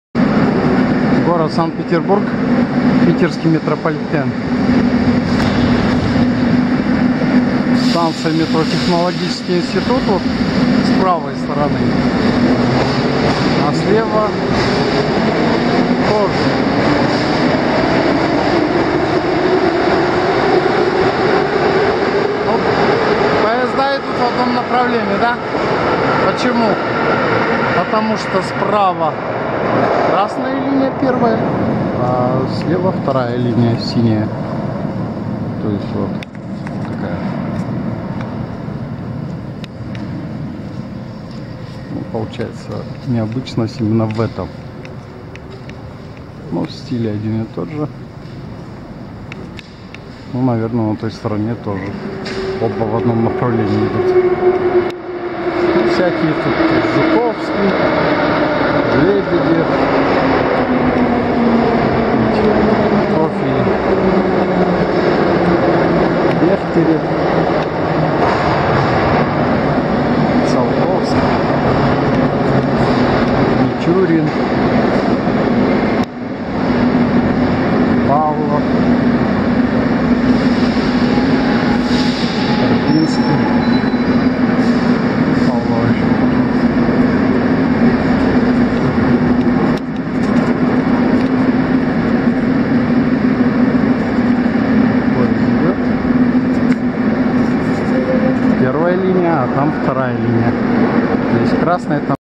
Станция Технологический институт | Питерский метрополитен
Станция Технологический институт в городе Санкт-Петербург на первой (Красной) линии Питерского метрополитена. Архитектура стильная, смотрится богато. Эта станция отличается тем, что здесь в одном тоннеле две линии совмещаются.